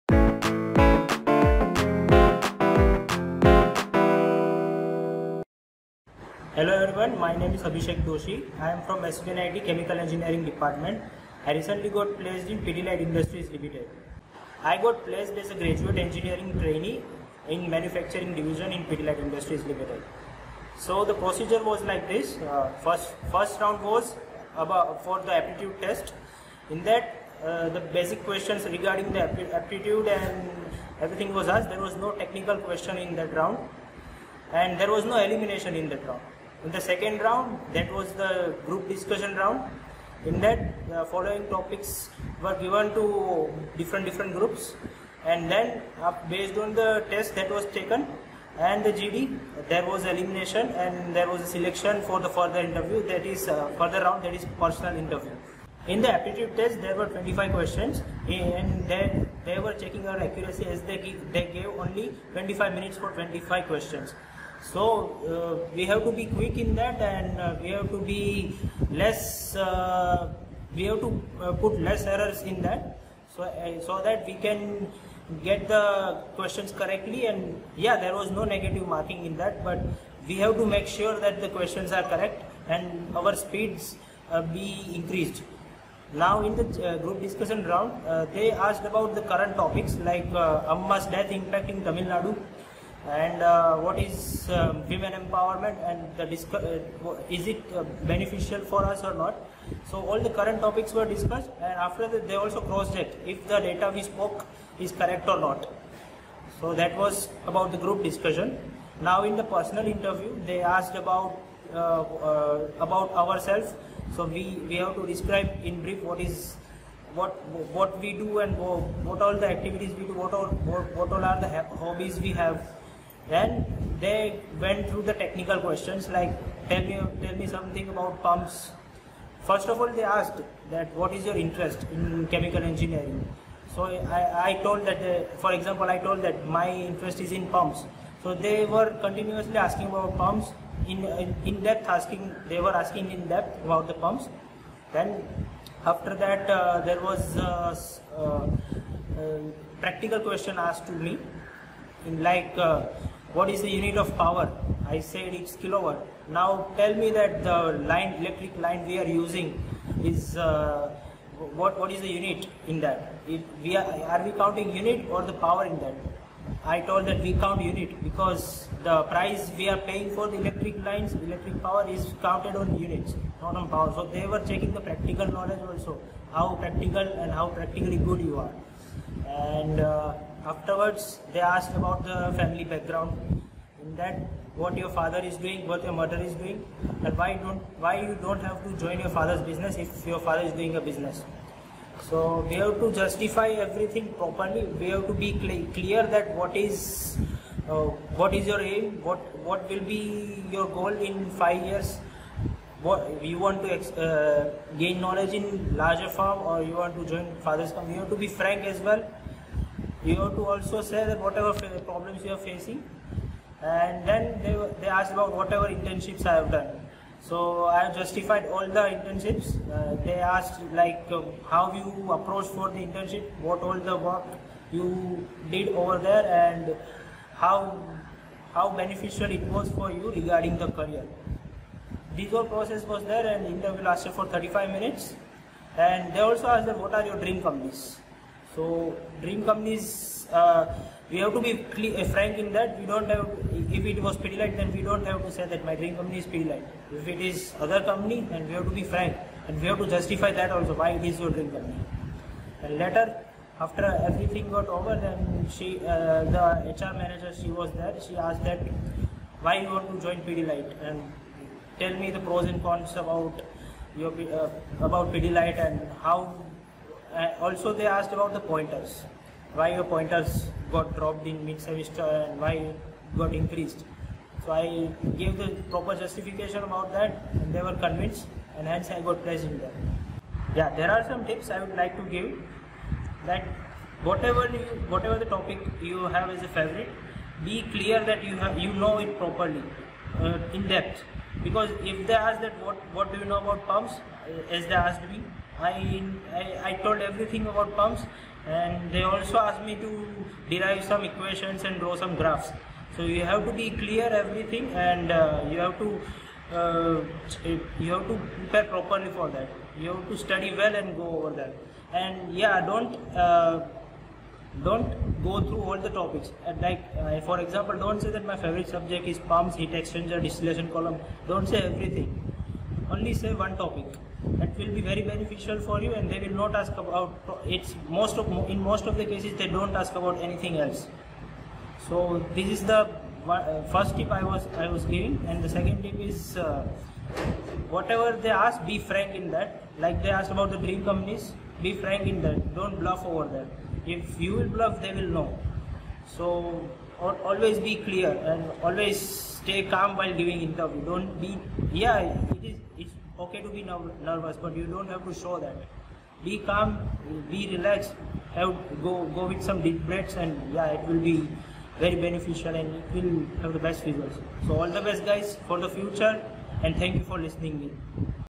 Pidilite : Interview Questions and Useful Tips.
This interview will not just throw light on Pidilite  recruitment procedure but will also give you loads of useful tips.